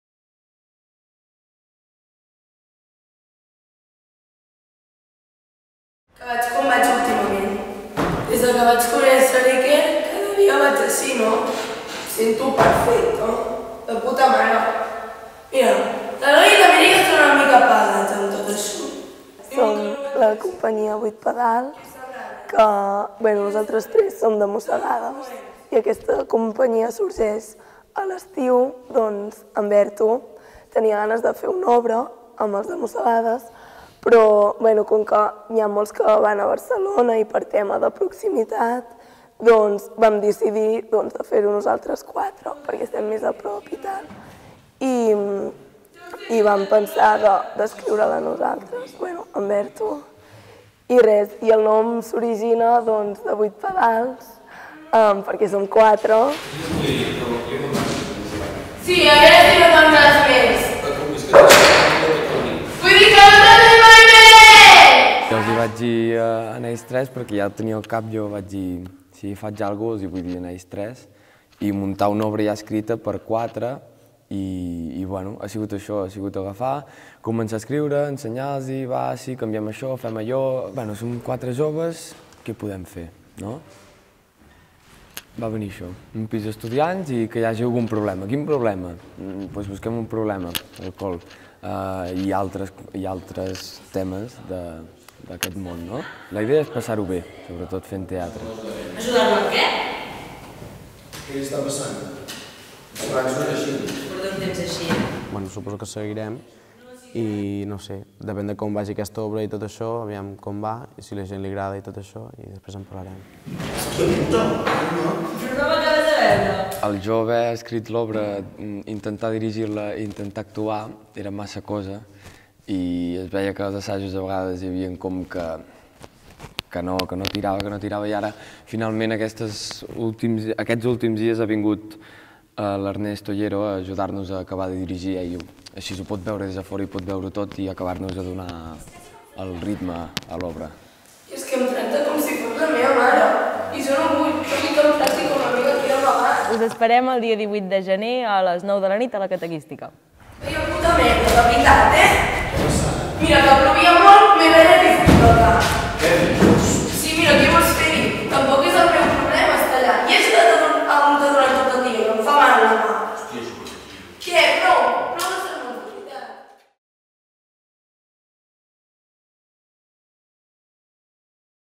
'Què és aquesta llum?'
La companyia '8 pedals' estrena la seva obra a la sala La Catequística de Figueres. Aquest grup sorgeix de 'A mossegades' i vol apropar el teatre al públic juvenil. L'obra té lloc en un pis d'estudiants, on una de les protagonistes té problemes amb l'alcohol, entre d'altres.